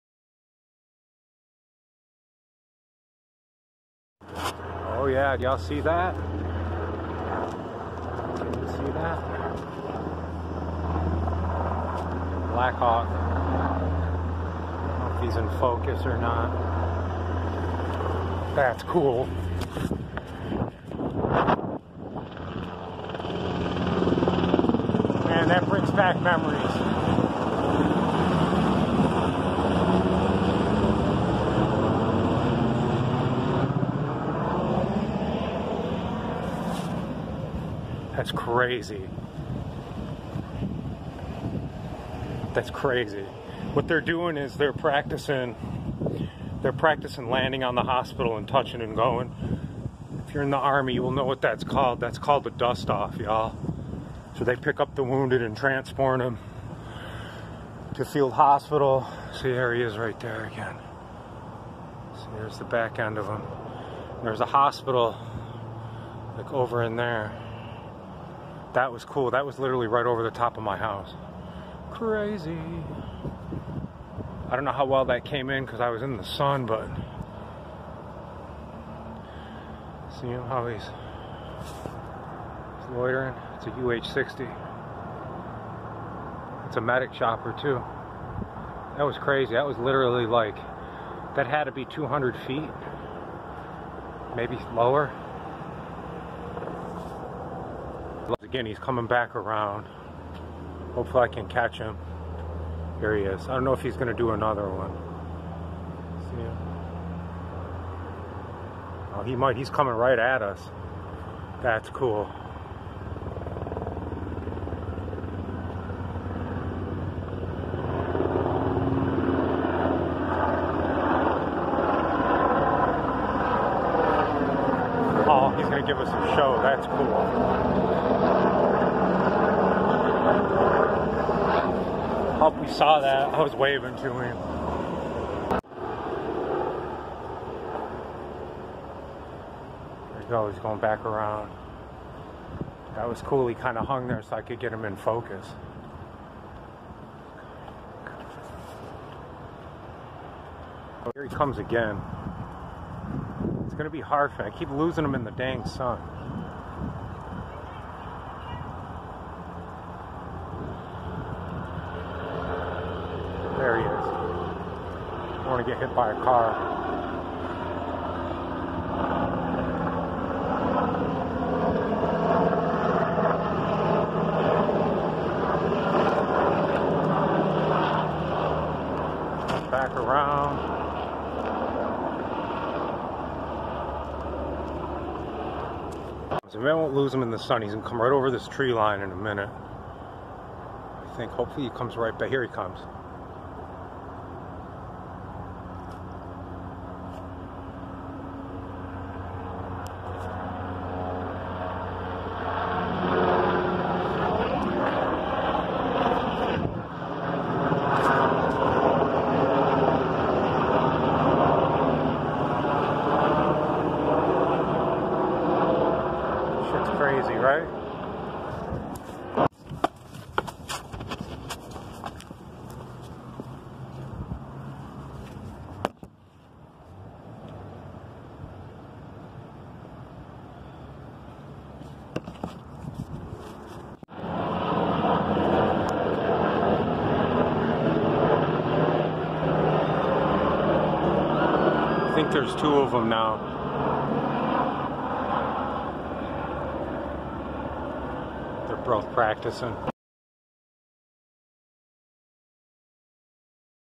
UH-60 (Blackhawk).. M.O.U.T. Dust-off maneuvers
M.O.U.T. Dust-off maneuvers.. In Syracuse NY.. It's nice to see a military presence.. I Kno I sleep better at night.